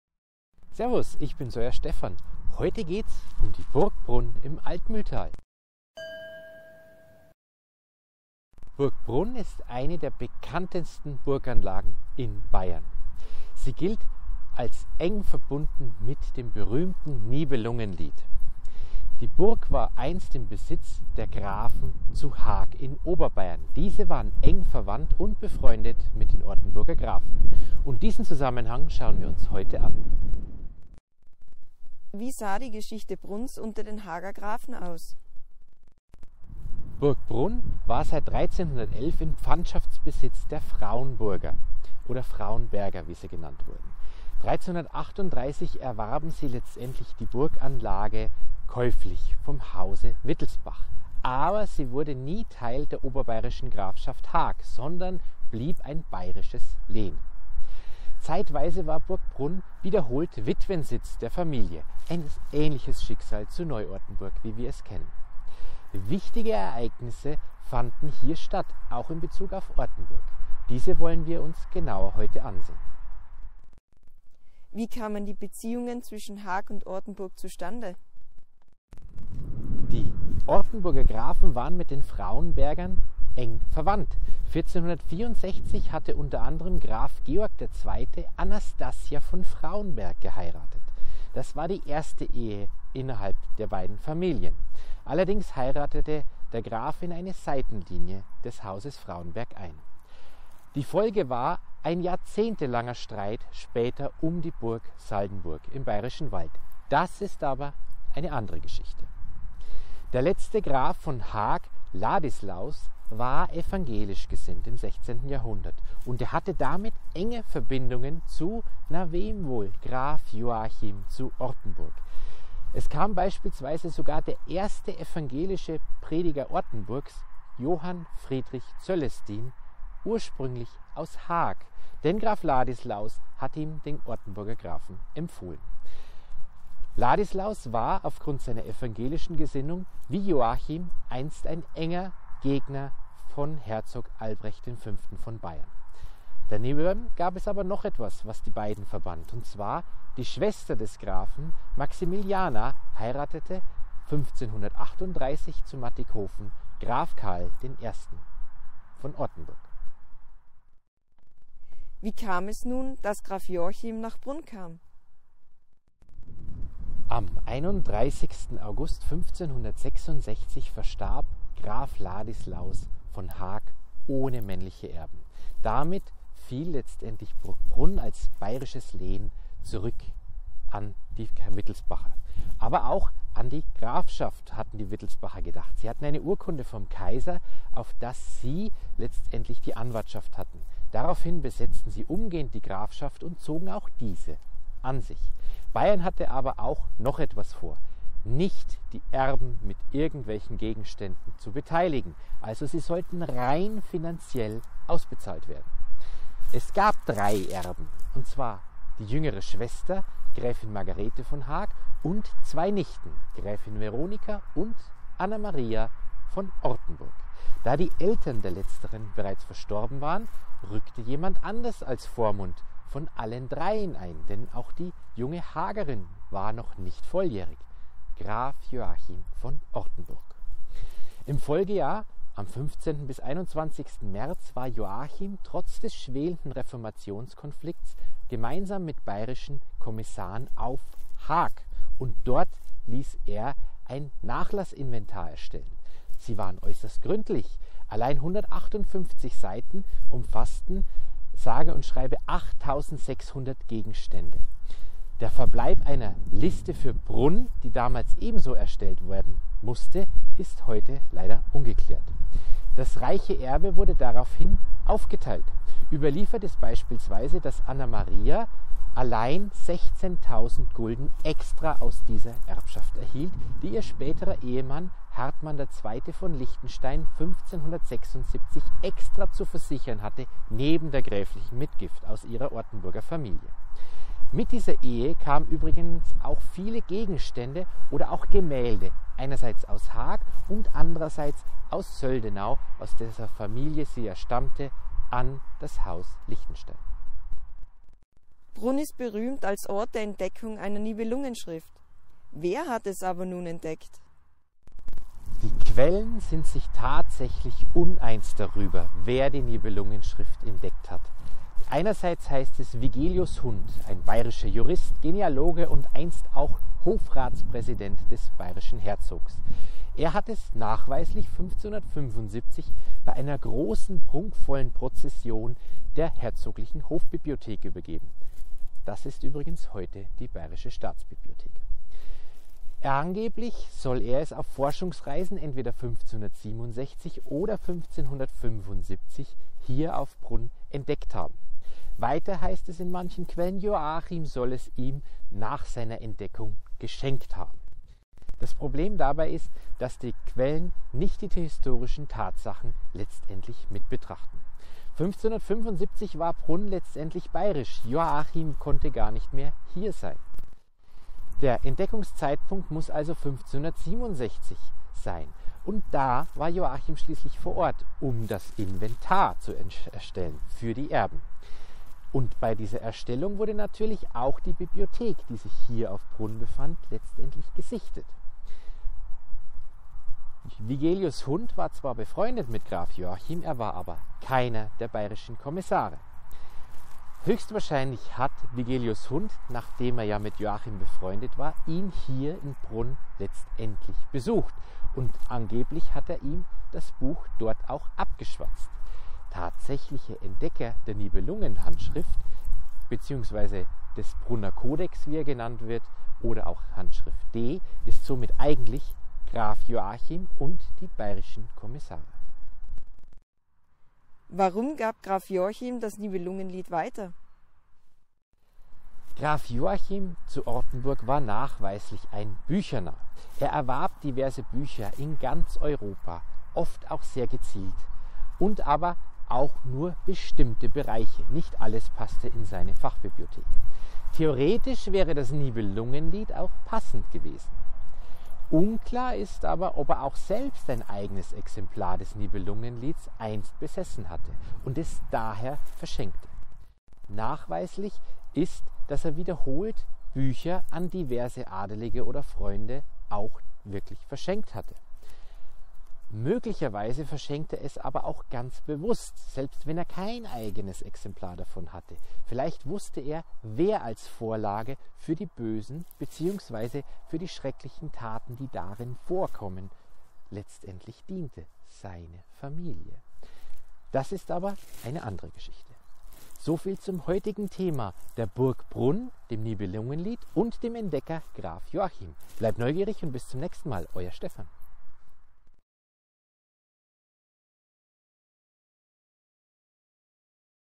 Ortenburgica unterwegs in Riedenburg: Graf Joachim als Mitentdecker des Nibelungenliedes?
Im Altmühltal befindet sich eine der bekanntesten Burgen in Bayern: die Burg Prunn. Sie war einst im Besitz der mit den Ortenburgern verwandten Grafen von Haag. Als der letzte Graf dieser Familie ohne männliche Erben verstarb, wurde Graf Joachim von Ortenburg als Vormund für die noch minderjährigen Erben eingesetzt. Beide Grafen teilten ihre evangelische Gesinnung. Wir beleuchten unter anderem das Geschehen rund um die Erstellung des Nachlassinventars und die Entdeckung einer Pergamenthandschrift des Nibelungenliedes.

Inhalt:
00:00 Einleitung
00:31 Burg Prunn unter den Haager Grafen
01:14 Beziehungen zwischen Haag und Ortenburg
02:41 Graf Joachim auf Burg Prunn
04:58 Der Entdecker der Nibelungenschrift
06:54 Das verschenkte Nibelungenlied
07:56 Signation

Kleine Anmerkung unsererseits: Wir entschuldigen uns für das stellenweise unruhige Bild bzw. etwaige Rausch- und Knackgeräusche. Der Wind war auf dieser Reise nicht ganz unser Freund.

Quellen:
Rudolf Münch, Das große Buch der Grafschaft Haag, Band 3, Die Zeit des Grafen Ladislaus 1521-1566, Haag in Oberbayern 1993, S. 4, 23, 34, 37, 82–85, 186, 198, 200 f., 220–222.
Rudolf Münch, Die Reichsgrafschaft Haag, Haag in Oberbayern 1980, S. 75, 87–89, 131, 149, 166 f.
Gerald Dobler, Grab und Grabmal des Grafen Ladislaus von Haag, in: Heimat am Inn, Beiträge zur Geschichte, Kunst und Kultur des Wasserburger Landes 35/36, Wasserburg 2016, S. 66.
Mathias Miller, Diß puech sollen meine nach komen mitt fleiss bewaren und auffheben ... Reichsgraf Joachim von Ortenburg und seine Bücher. Rekonstruktion seiner Bibliothek (Ortenburger Schriften zur Geschichte 2), Ortenburg 2020, S. 26, 43, 55 Nr. H38.
Erich Petzet, Die Deutschen Pergament-Handschriften, Nr. 1 – 200 der Staatsbibliothek in München, München 1920, S. 54 f. Cgm. 31.
Friedrich Hausmann, Die Grafen zu Ortenburg und ihre Vorfahren im Mannesstamm, die Spanheimer in Kärnten, Sachsen und Bayern, sowie deren Nebenlinien. Ein genealogischer Überblick, in: Ostbairische Grenzmarken 36, Passau 1994, S. 9–62, hier S. 32
Ludwig Freiherr von und zu Fraunberg, Erläuterungen der Herren von Fraunberg zum Hag und zu Fraunberg, in: Frankfurter Blätter für Familiengeschichte, Jahrgang 6, November 1913, Heft 11, S. 162–170, hier S. 163 Nr. 29 (1405).
BayHStA Ortenburg-Archiv Akte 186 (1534–1540).
BayHStA Ortenburg-Archiv Akte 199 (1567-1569).
BayHStA Grafschaft Ortenburg Urkunde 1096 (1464 XII 4).
BayHStA Grafschaft Ortenburg Urkunde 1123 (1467 IV 21).
BayHStA Grafschaft Ortenburg Urkunde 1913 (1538 VIII 10).
BayHStA Grafschaft Ortenburg Urkunde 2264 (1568 V 15).
BayHStA Grafschaft Ortenburg Urkunde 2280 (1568 X 29).
BayHStA Grafschaft Ortenburg Urkunde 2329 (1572 IX 29).
BayHStA Kurbaiern Urkunde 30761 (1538 VIII 10).